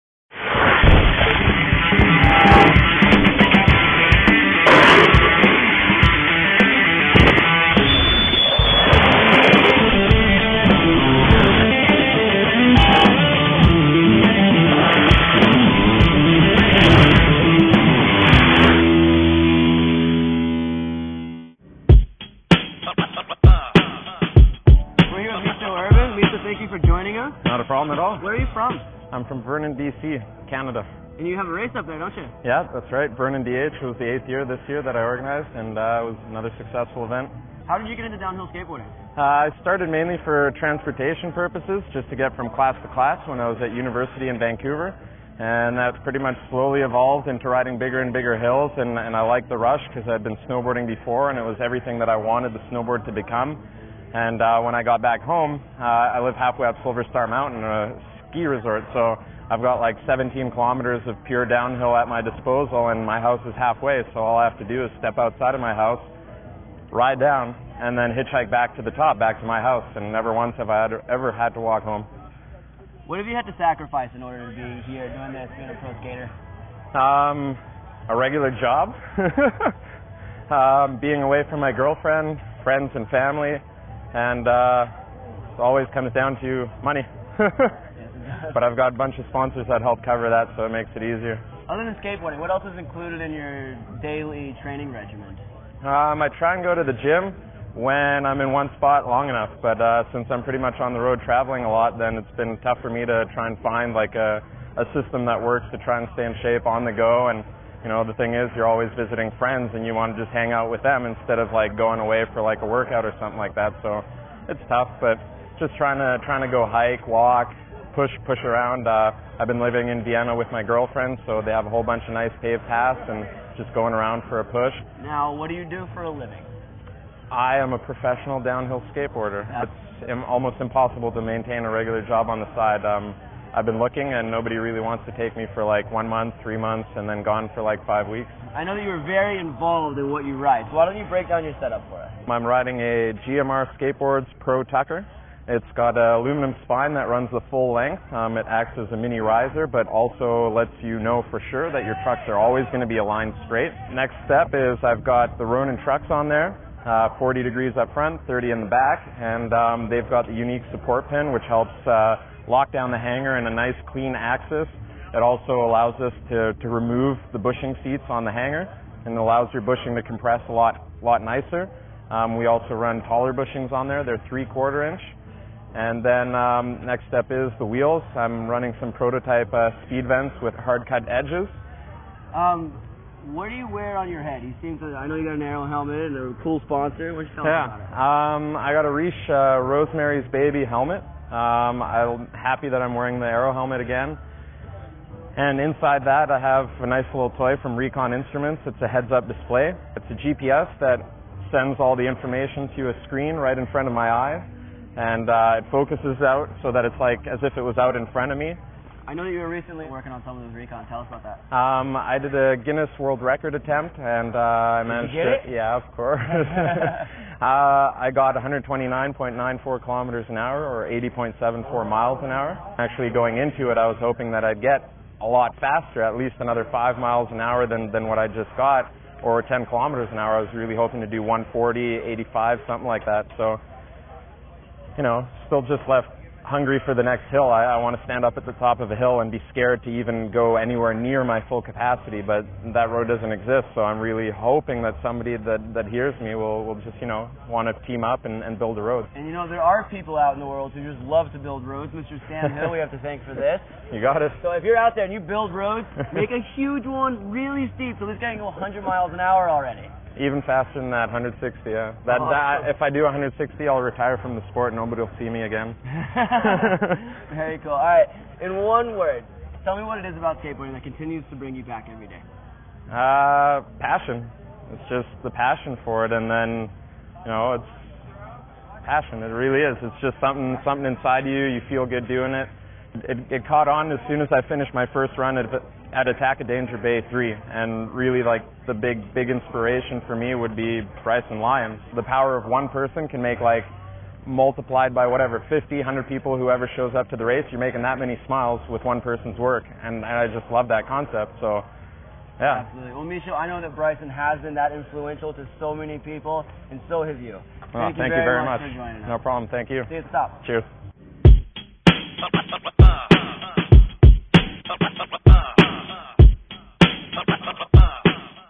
Rider interview Mischo Erban at the Maryhill Festival of Speed - Push Culture News
As the official media partner of the Maryhill Festival of Speed this year we are bringing you even more great content then ever before.  We brought Mischo Erban, the fastest skater in the world to the news desk for an extended interview.  Check out all Mischo had to say about his new record and the gear he used to get it.

For the best in Longboarding News - Stay up to date so we can keep you informed!